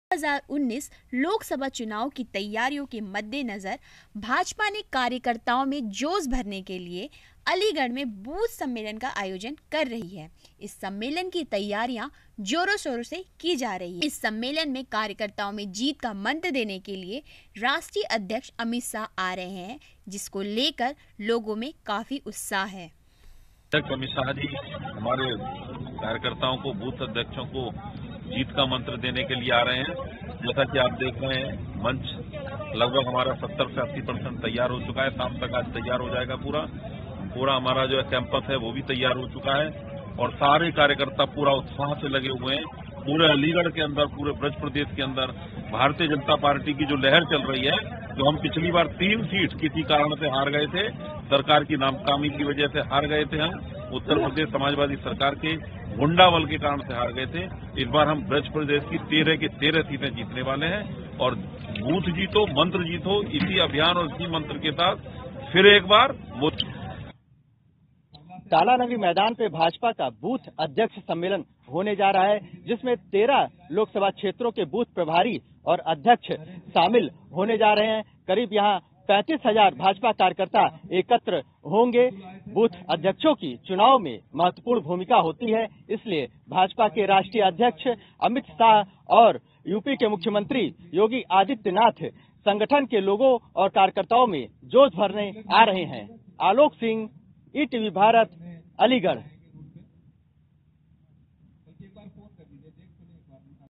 भाजपा बूथ सम्मेलन: ब्रज क्षेत्र की 13 लोकसभा सीट जीतने का मंत्र देंगे अमित शाह
अलीगढ़ : 2019 लोकसभा चुनाव की तैयारियों के मद्देनजर भाजपा ने कार्यकर्ताओं में जोश भरने के लिए अलीगढ़ में बूथ सम्मेलन का आयोजन कर रही है. इस सम्मेलन की तैयारियां जोर-शोर से की जा रही है. रामघाट रोड पर गड्ढा युक्त सड़क को पाटा जा रहा है, जहां पहले झाड़ू नहीं लगाई गई. वहां सड़क पर झाड़ू लगाई जा रही है. साथ ही रामघाट रोड को भगवा कलर से सजा दिया गया है.